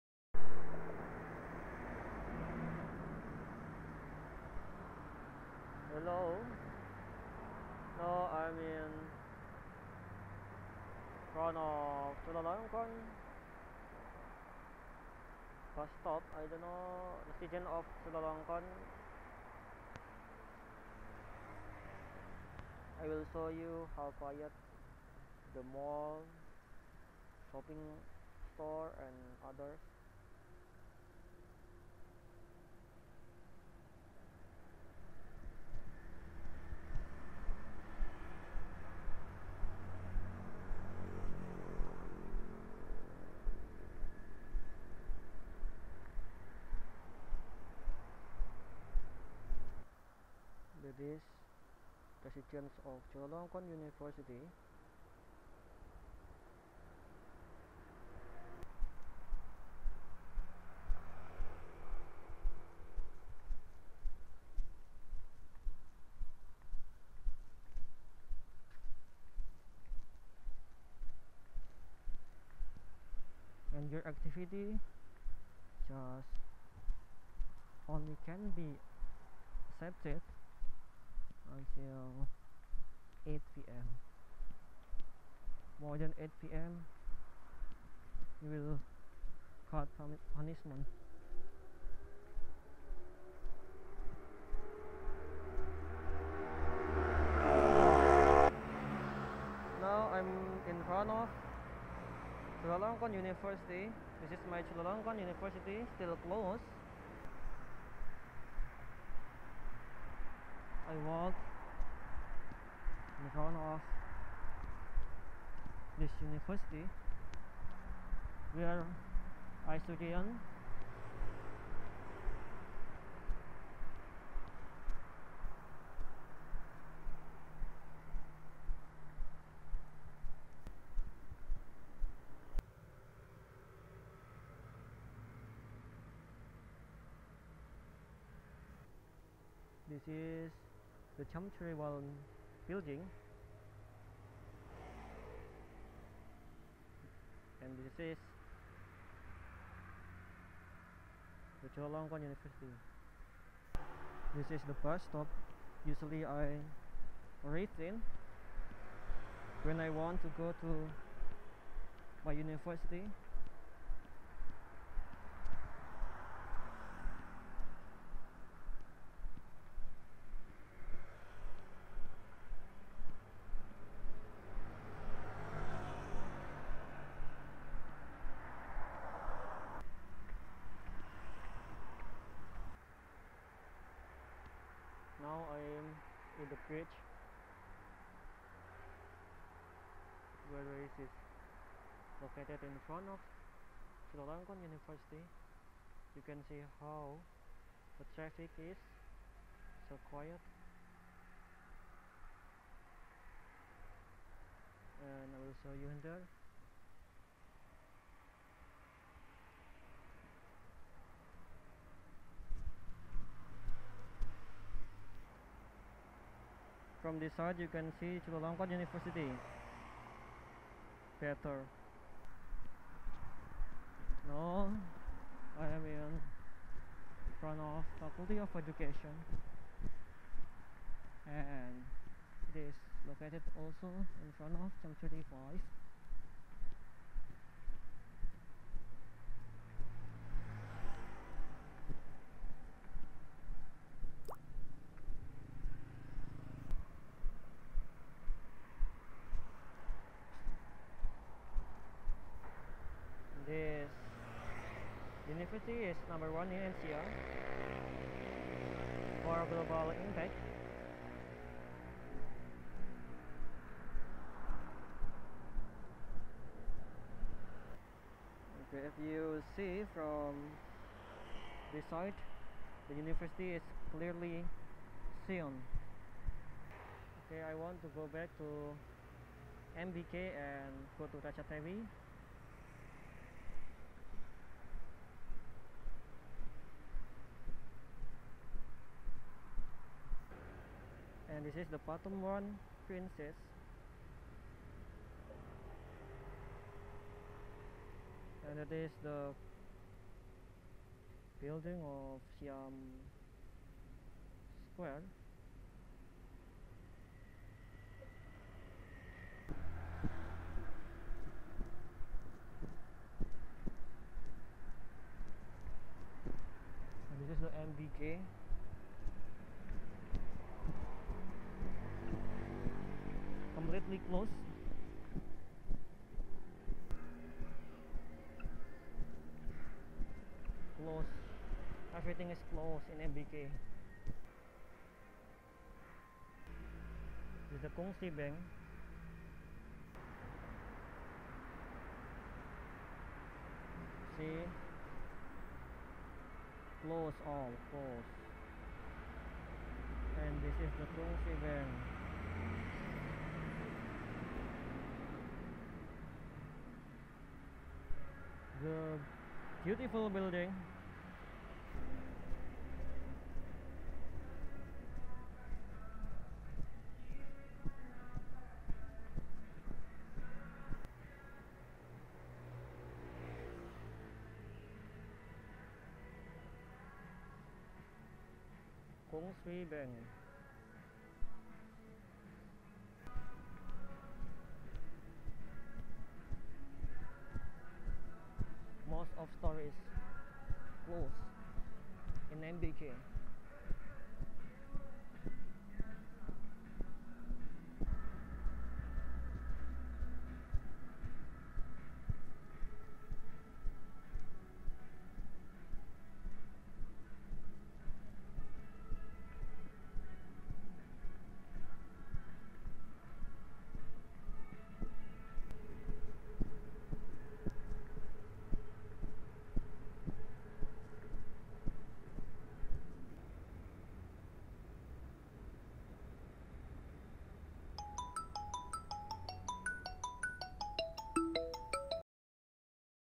Restrictions Extended since 12 July Closing University, Shopping Mall etc
Closing, restrictions, and rule can be read from the official website of Thailand

Restrictions
Restrictions
Restrictions
Closure
Closure
Closure

Recorded by: Xiaomi Redmi Note 8
Mode: 4K